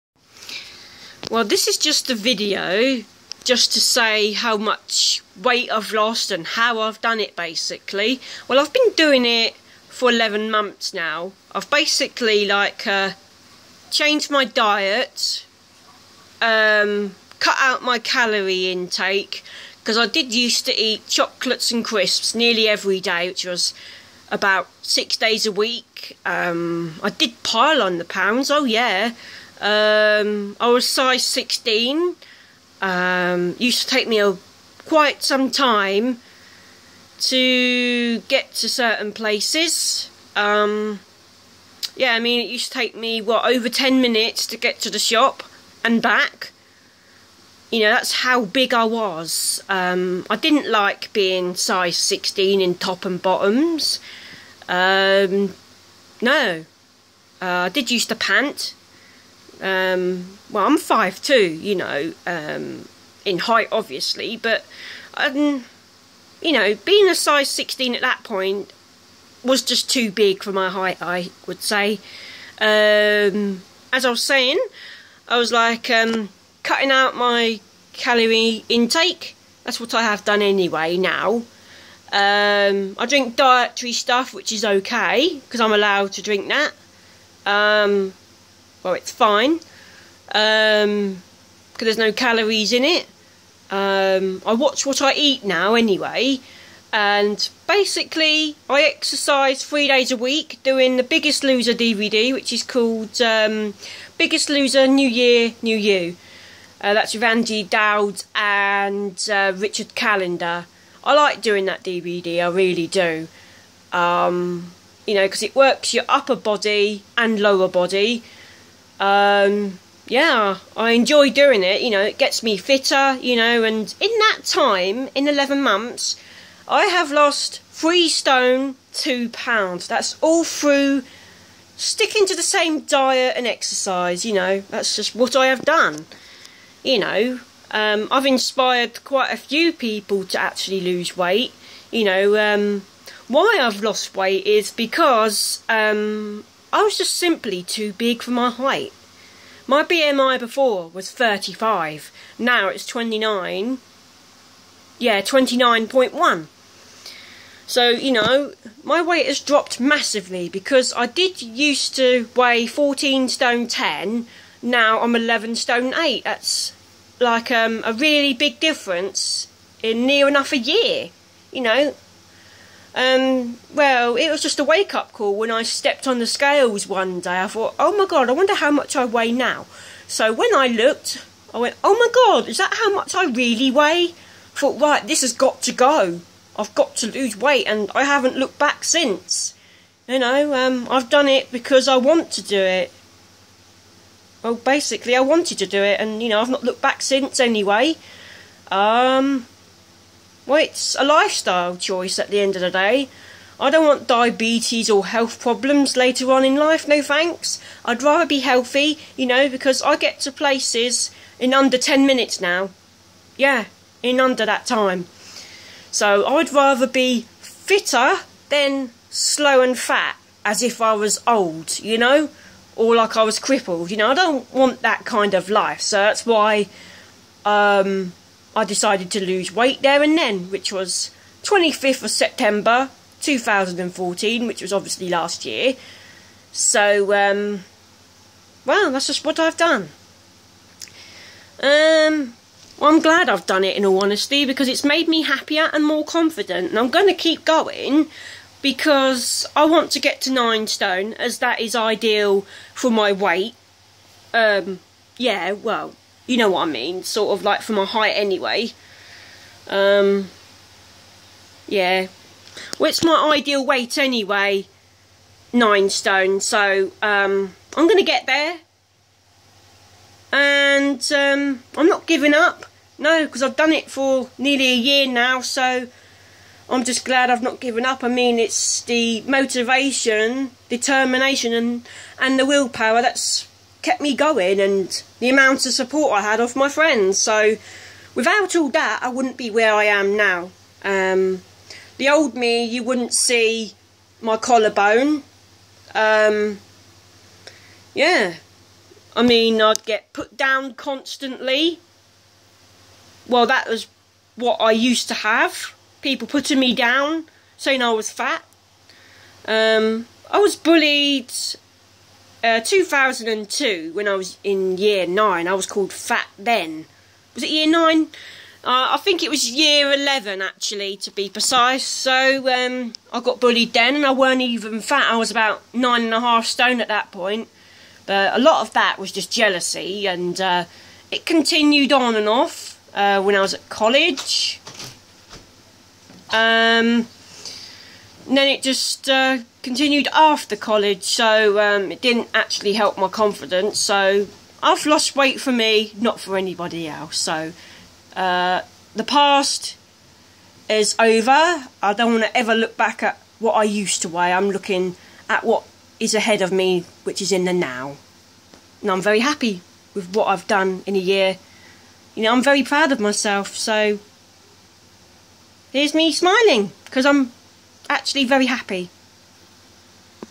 My weight loss
Video to describe how I lost weight and why